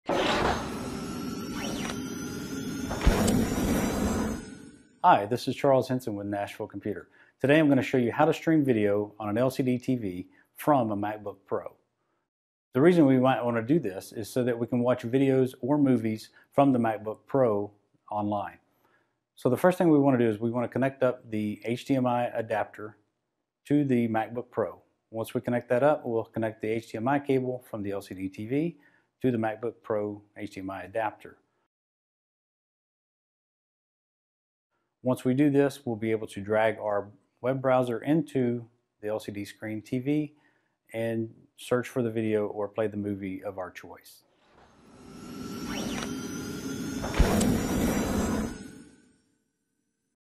How to Stream Video From a MacBook to an LCD TV
How to Stream Video From a MacBook to an LCD TV. Part of the series: Computer Hardware Help & More. Streaming video form a MacBook to an LCD TV is something that you do with the help of your home network. Stream video from a MacBook to an LCD television with help from a technology strategist and cloud expert for small-to-medium businesses in this free video clip.